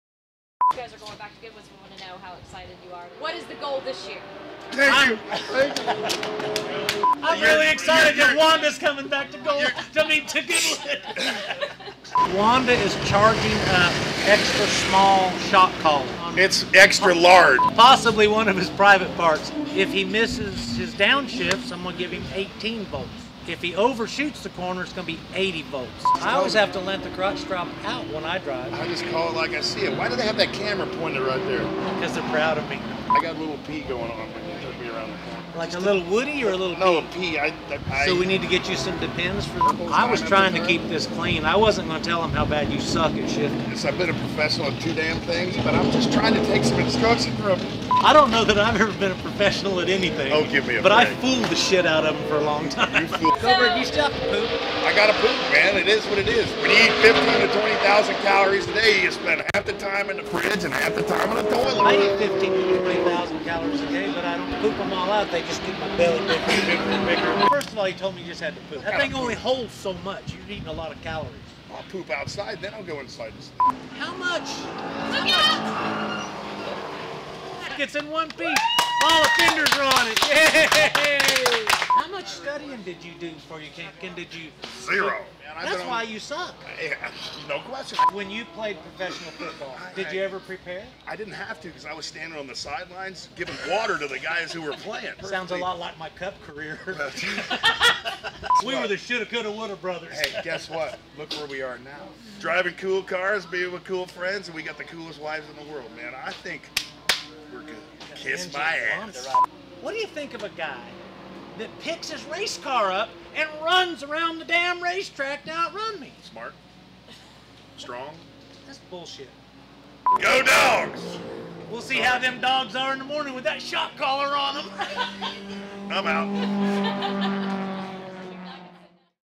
Mike Skinner - Bill Goldberg Sebring Outtakes
Mike and Bill funny out takes from the 2017 Sebring Track Day at Sebring International in November.